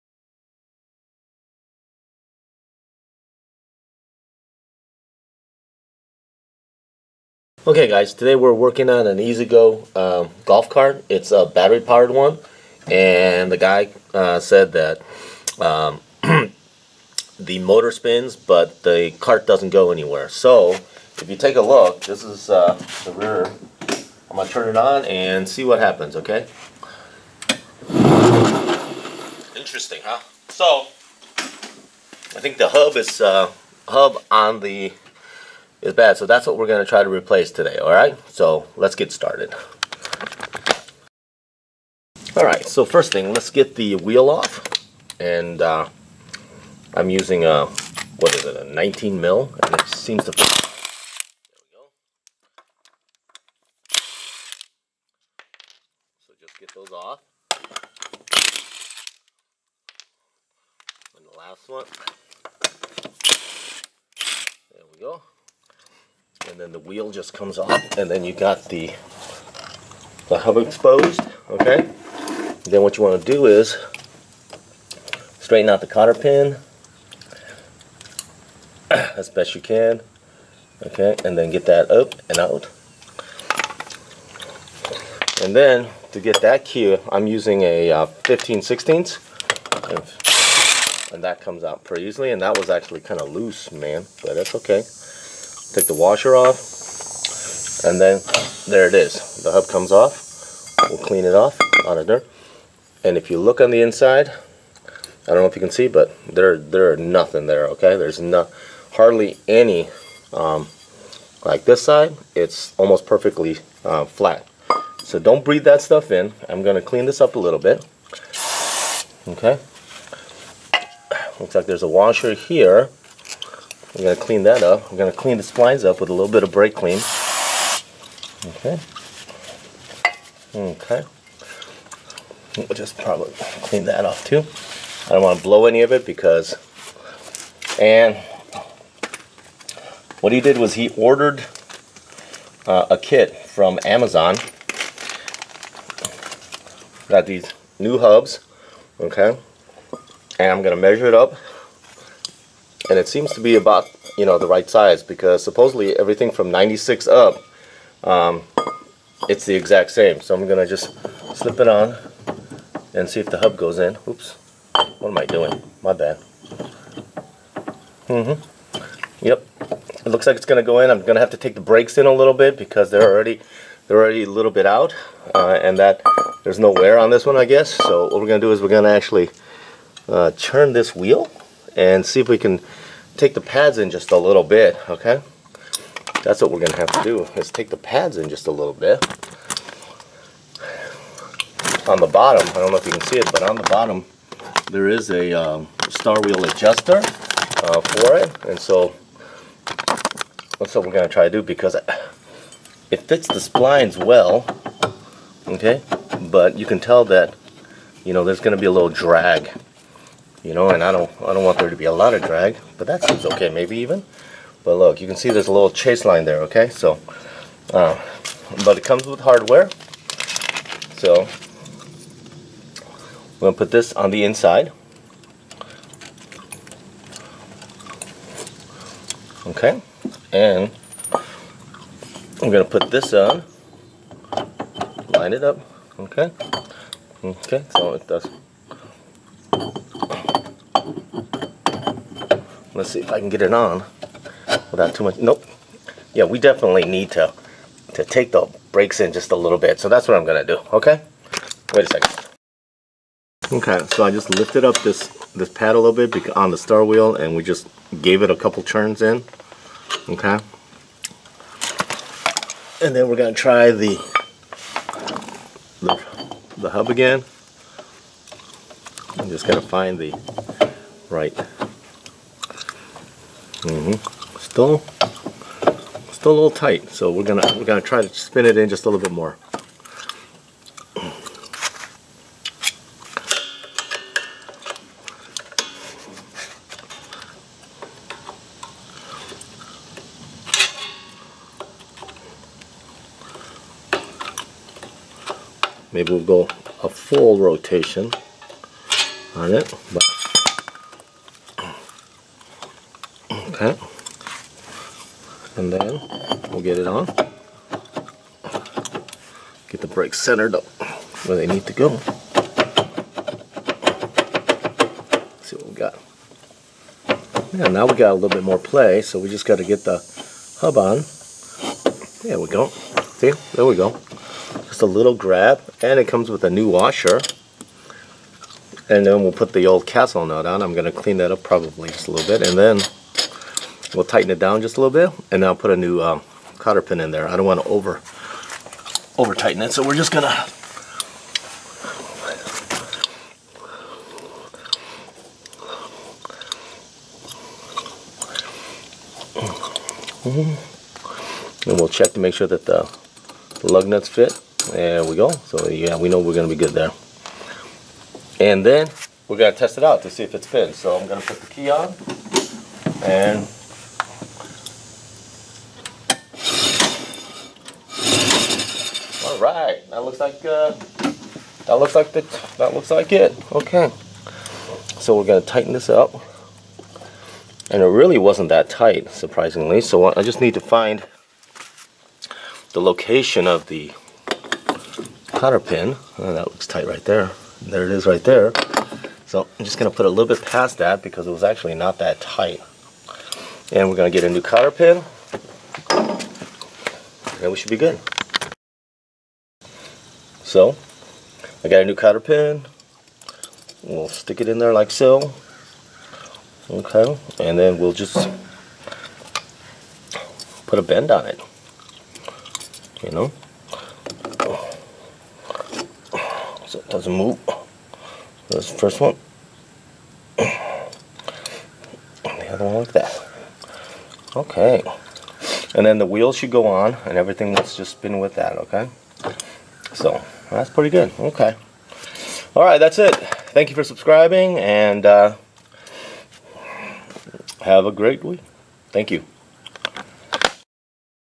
EZGO rear hub replacement
Owner complained that the motor could be heard, but the cart would not move.  Removing the wheel covers showed why.  This seems to be a common problem on these EZGO carts and the part is the same for model years 1982 and up for eclectic models.  Parts were ordered from Amazon, as the local golf shop didn't have any in stock.  Works great.